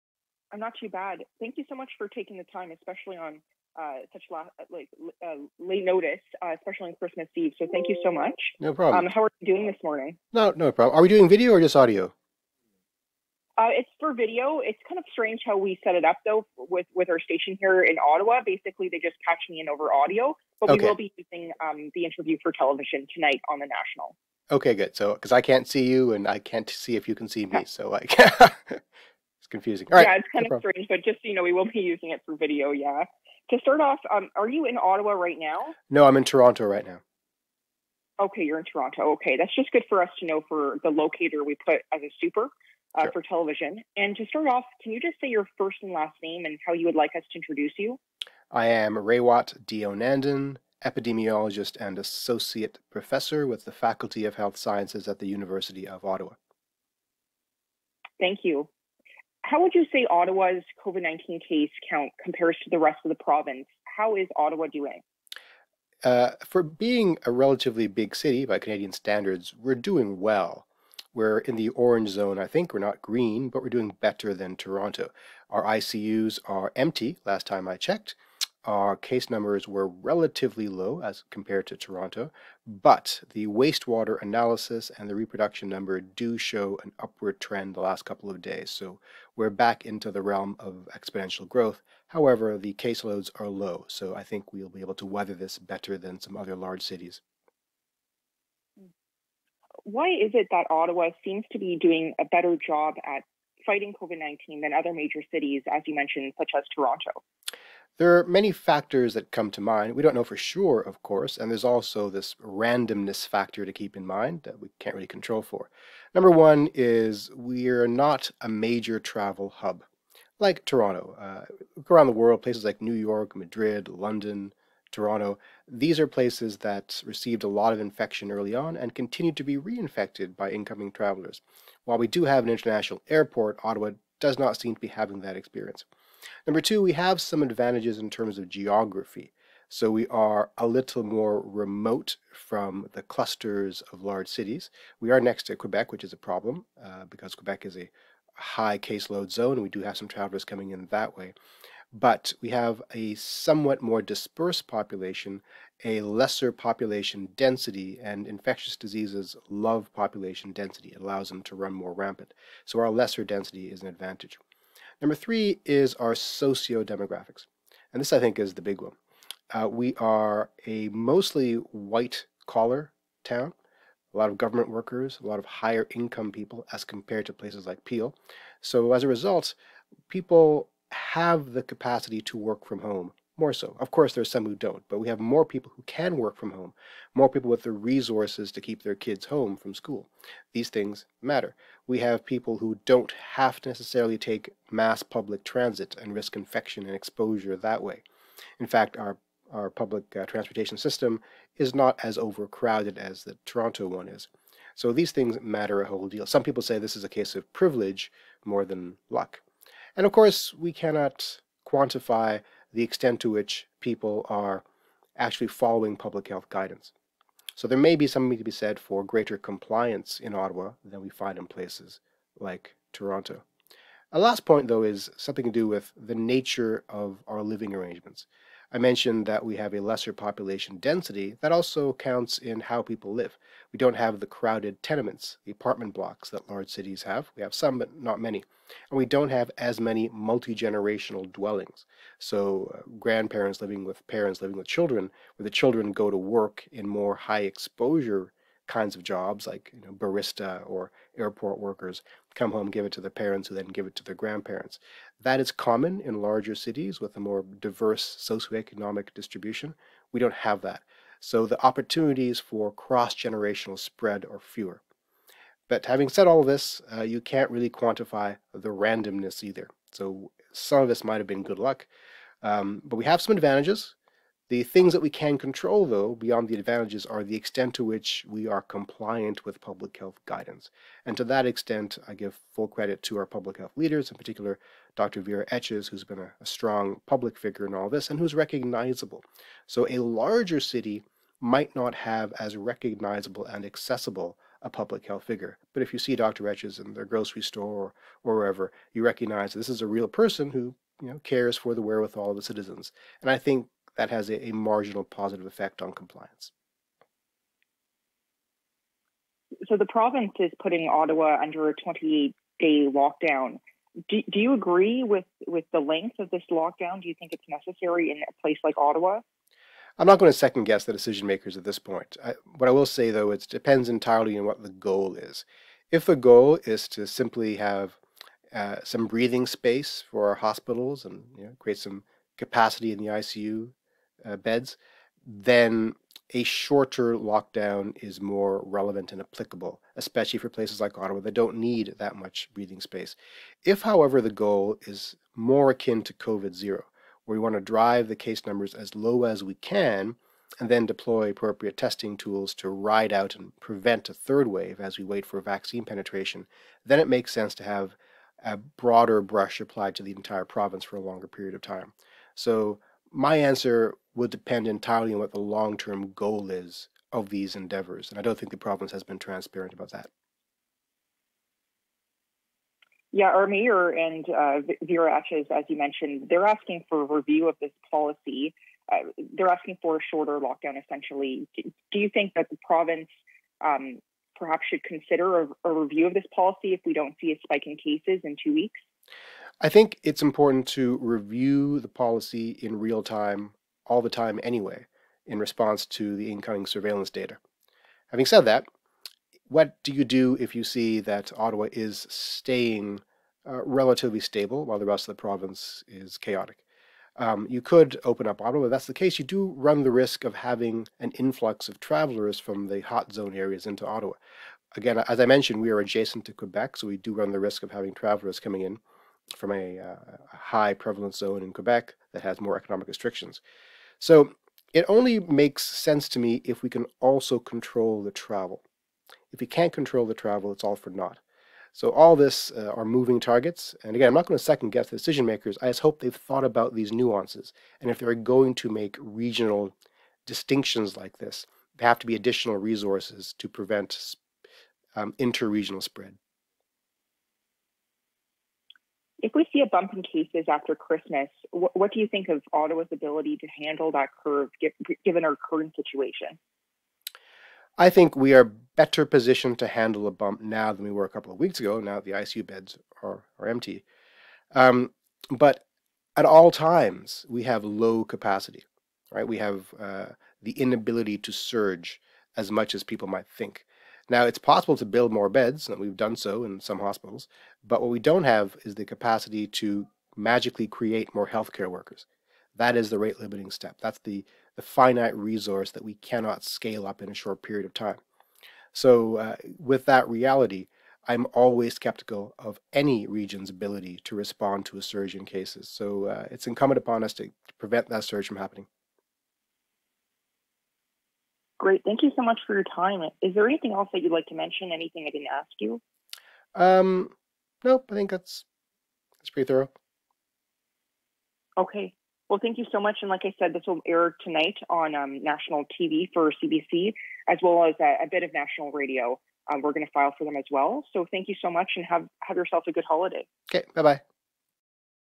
Dr Raywat Deonandan on CBC News, Dec 24 2020 (raw feed)
Interview by Olivia Stefanovich for CBC News. This audio was recorded on my computer. Portions of the audio and video will be broadcast on national radio and TV. But this is the entirety of the interview.